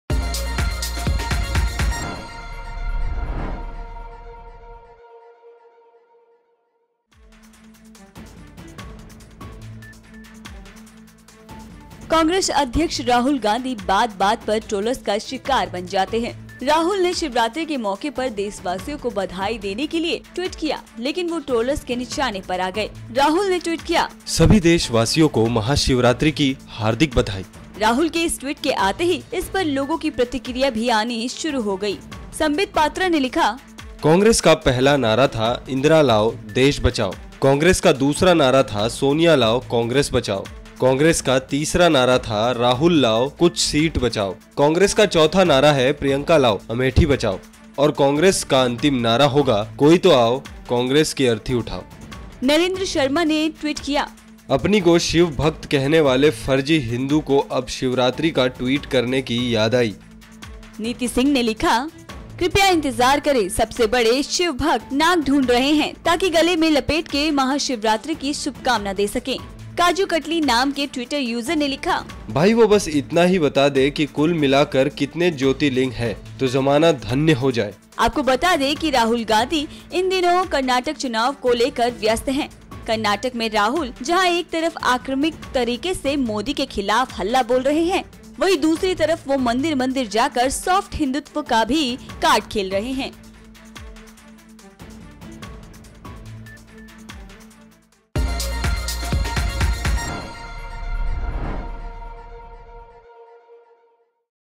Rahul Gandhi ने Twitter पर दी Shivratri की बधाई, लोगों ने जमकर किया Troll
Rahul Gandhi अक्सर Twitter पर Troll होते रहते हैं... Rahul एक बार फिर Twitter पर Troll हो रहे हैं और वजह है उनके द्वारा Shivratri पर किया गया Tweet.